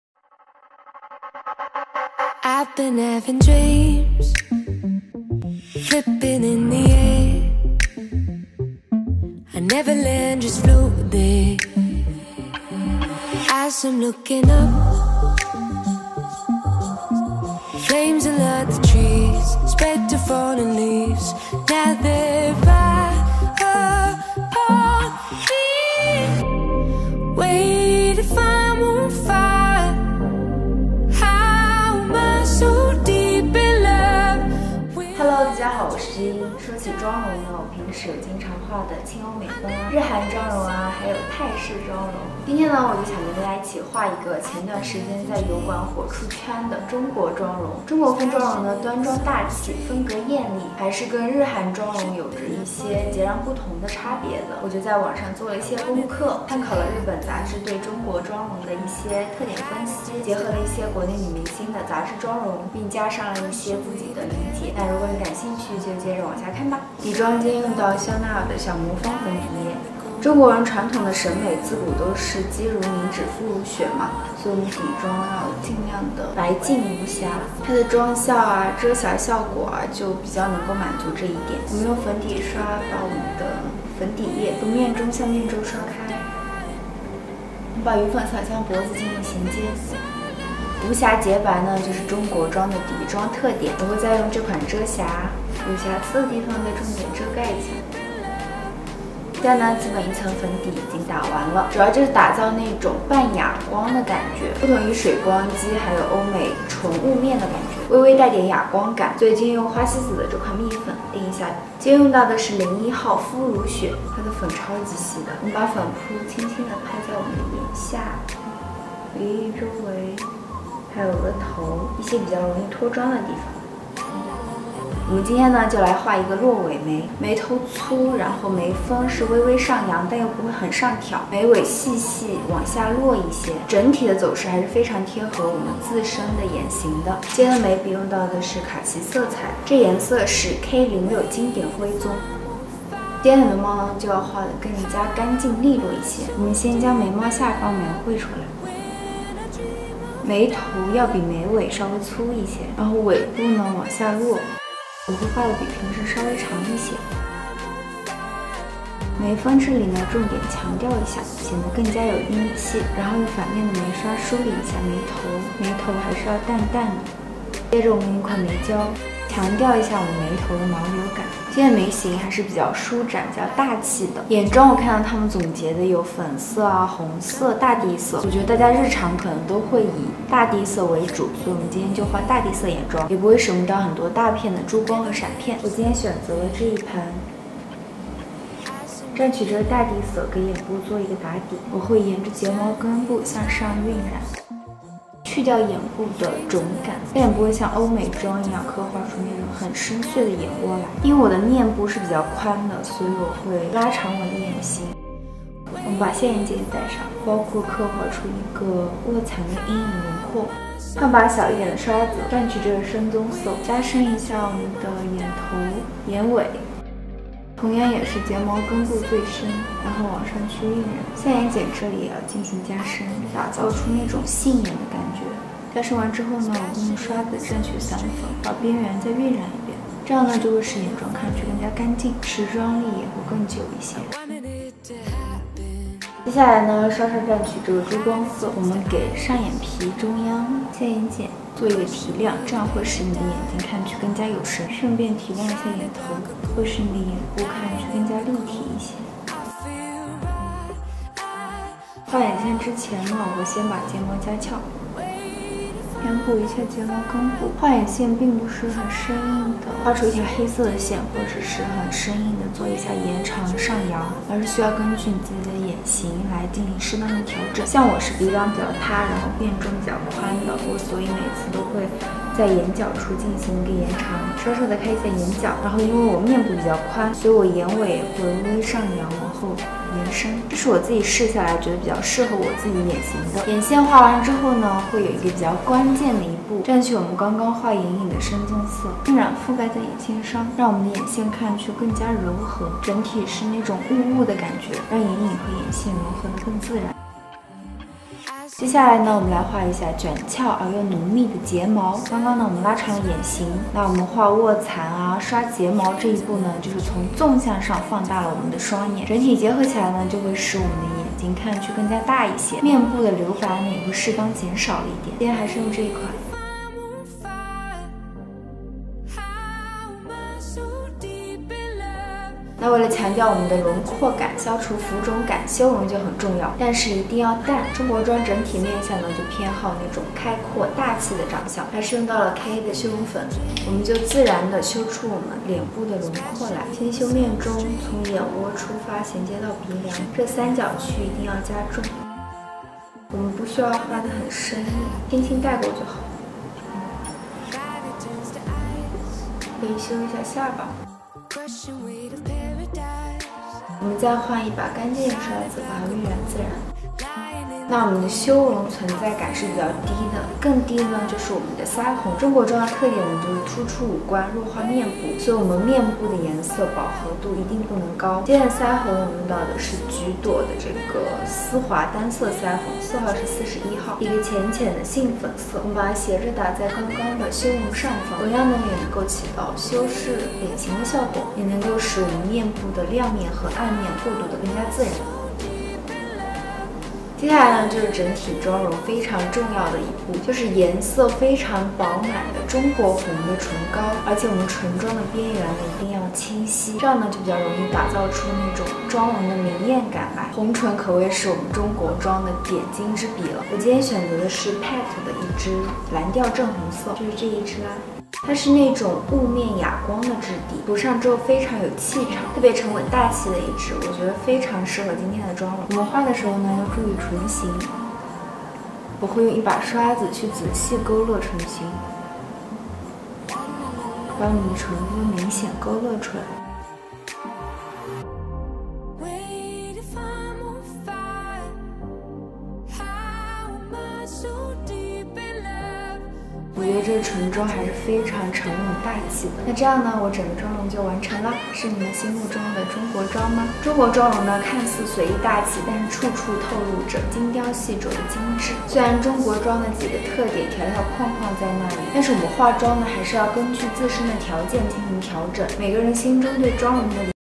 风靡外网的中国妆是什么样？沉稳大气国风妆|精致高级感
油管上爆火的中国妆，中式审美也走向世界啦！
明艳大气的中国妆，化上就有气场全开的魔力。
希望每个女孩都能找到自己的特点，化出自己眼中的中国妆！